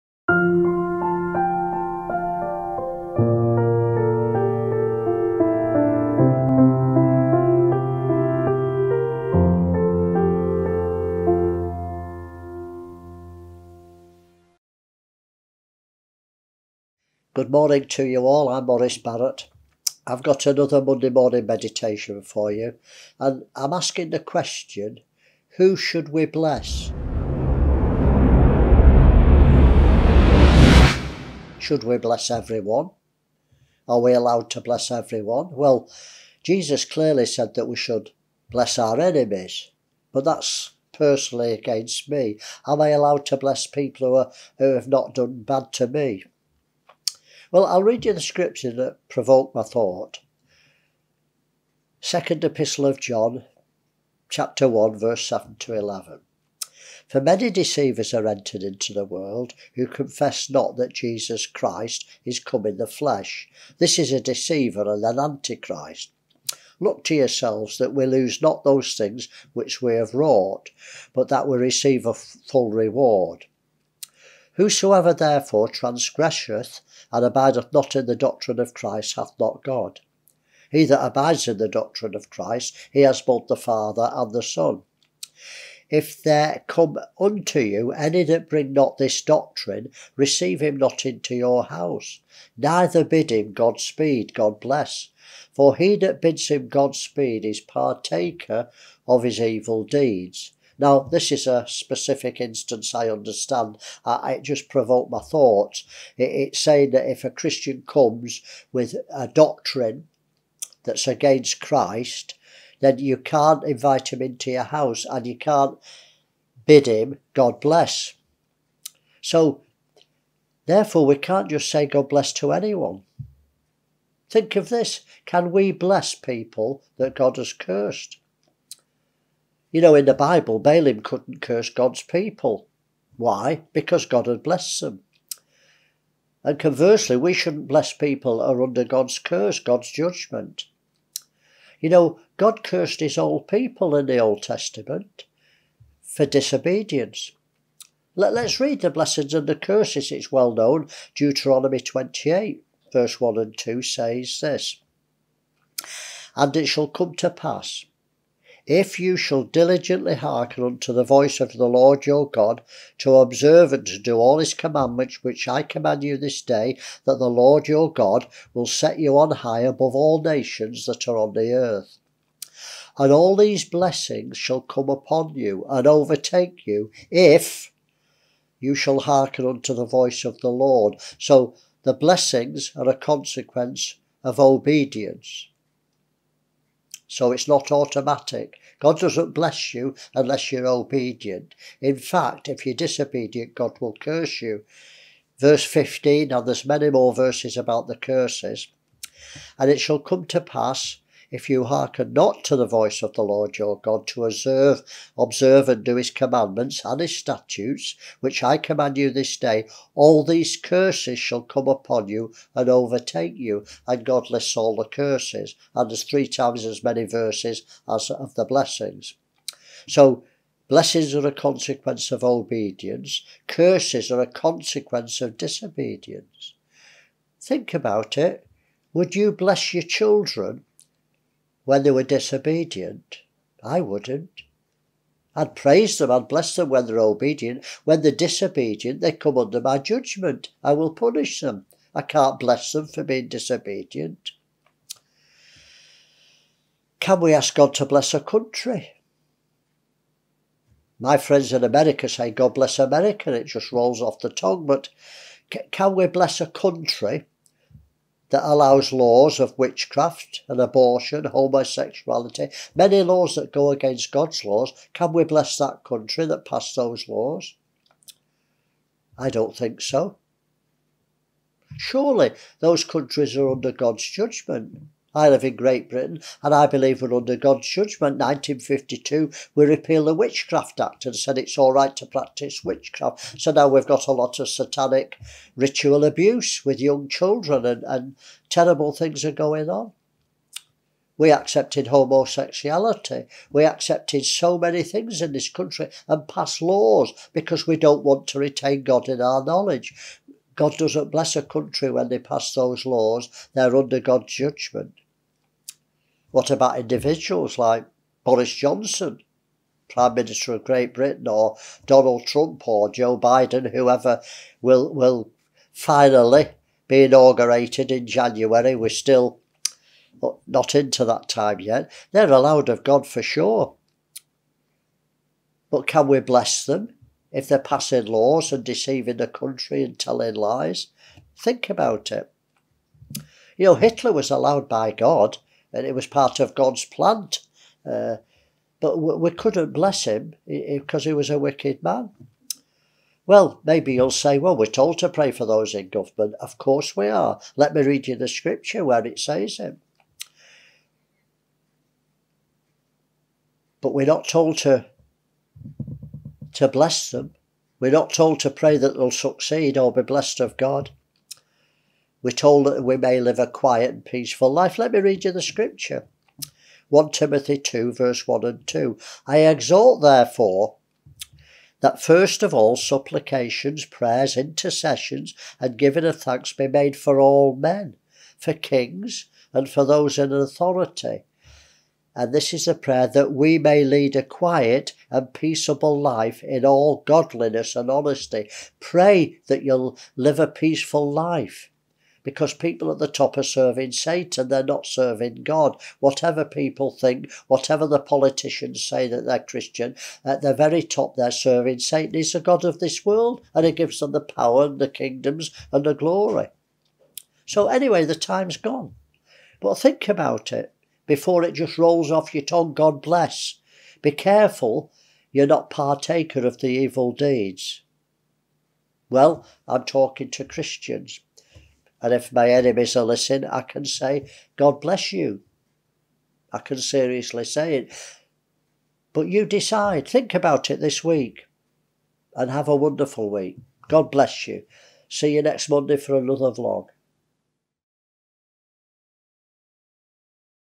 64 Who Should We Bless
Can we just bless everyone. Maurice doesn't think so